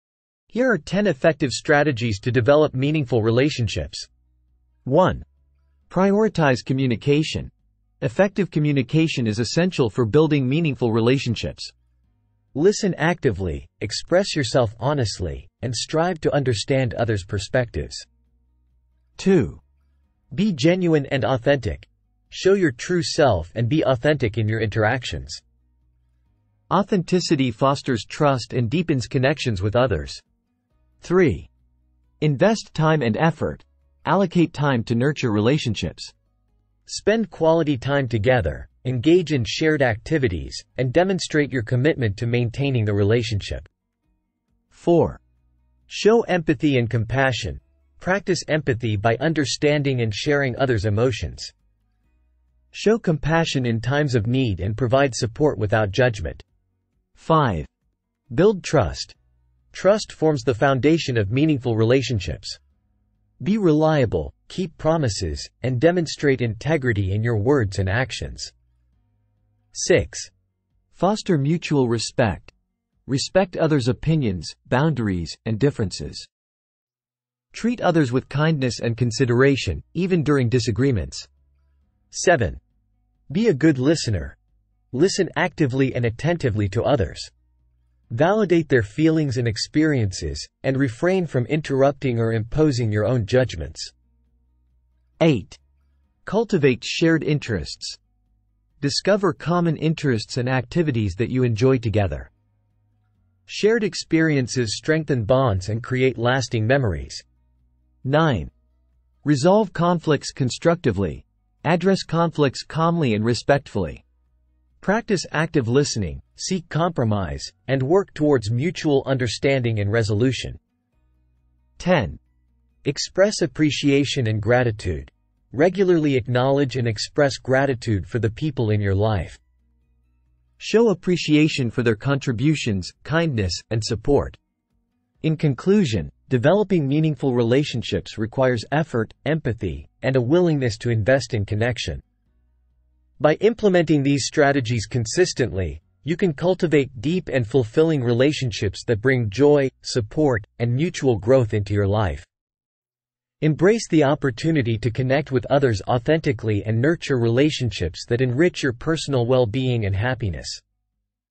Top 10 Ways to Develop Meaningful Relationships
Enhance the quality of your relationships with practical tips and strategies that foster connection, communication, and mutual understanding.